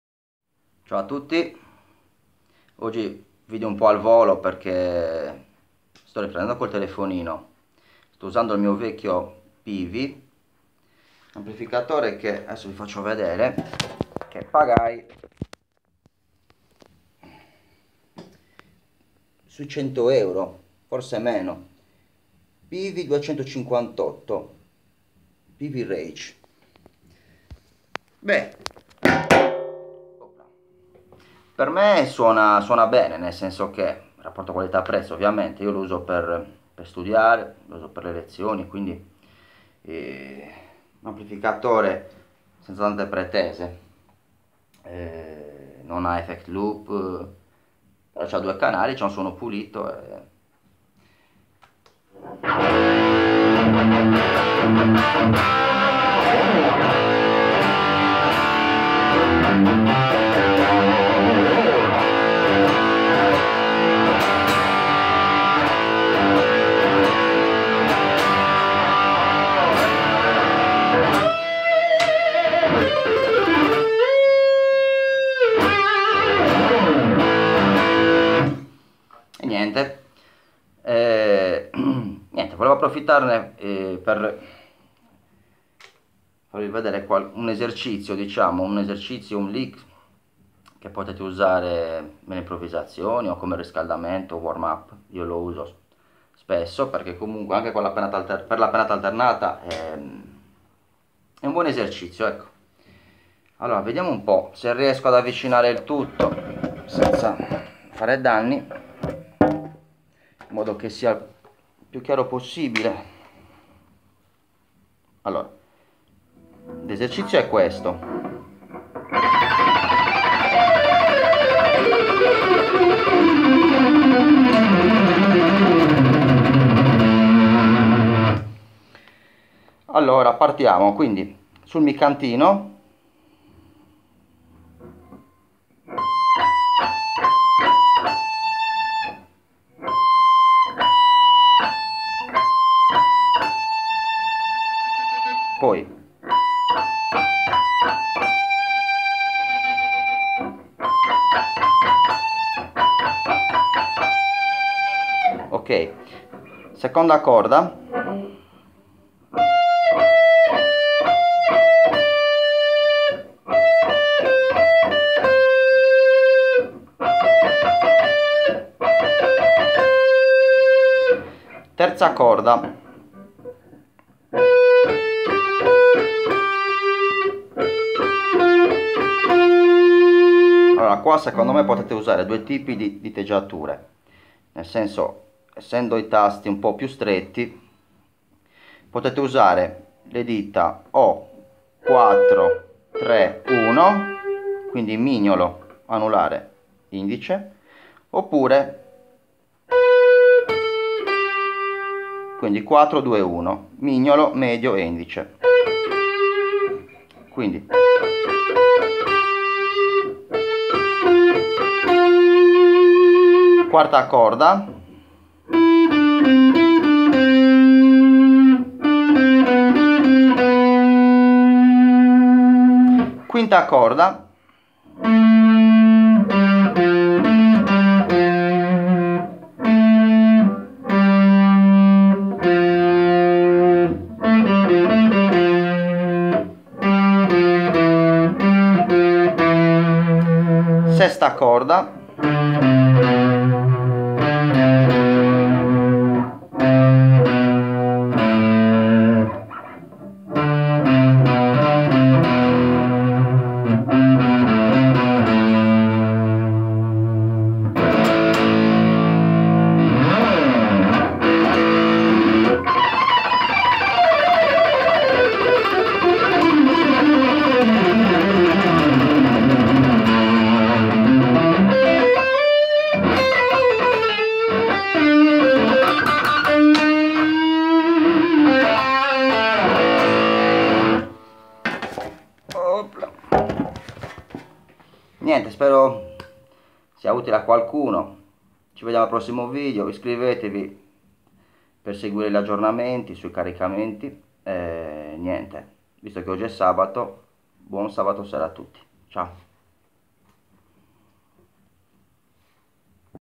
Guitar Lick #1 - Andrea Maccianti
Esercizio sulla pennata alternata sulla scala di La Minore.